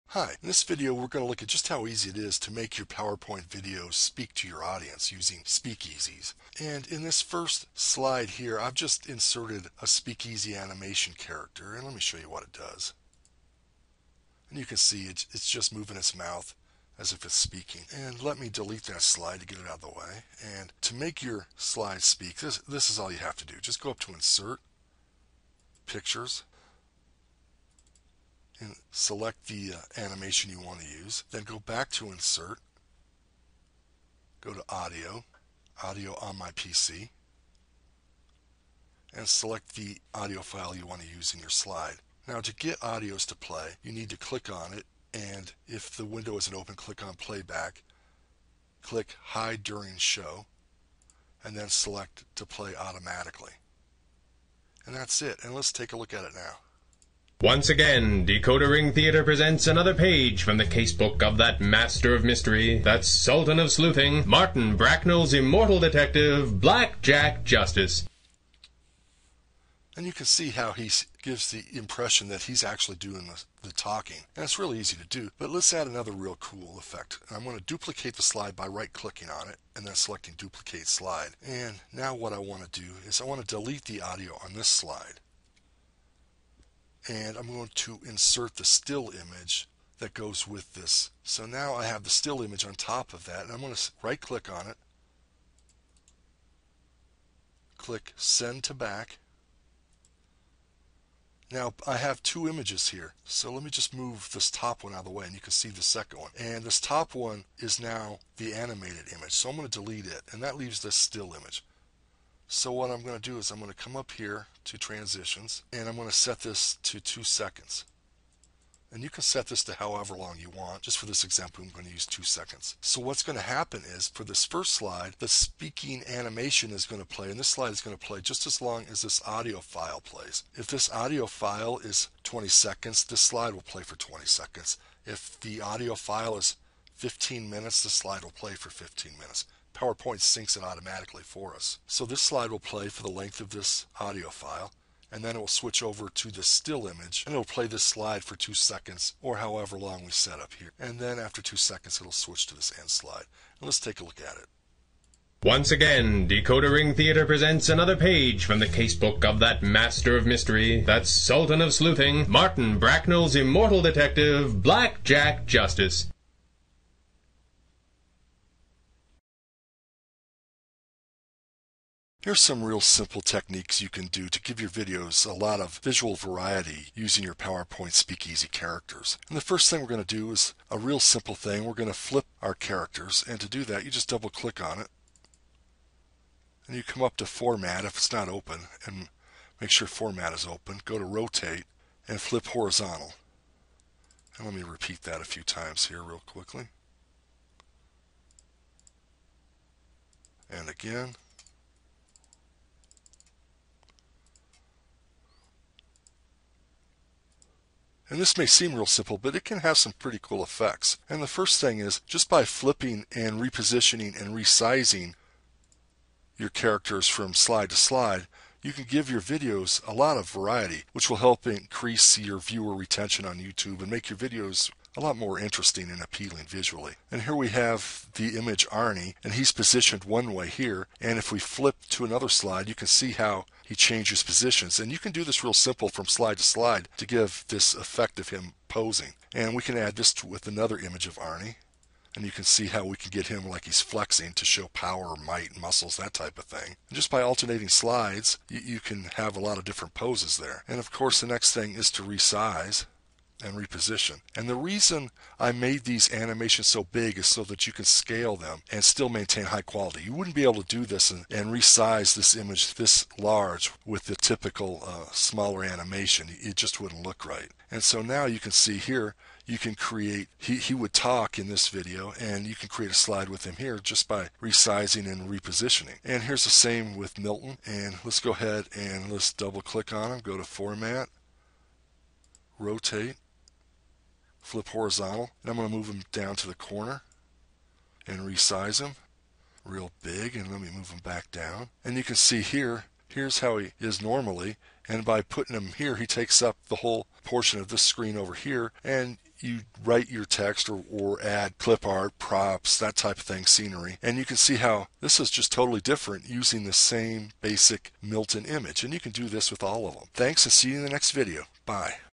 PowerPoint Speak Easies how to, tips and tricks
Quick video showing how easy it is to make your PowerPoint videos talk to your audience using Speak Easies characters.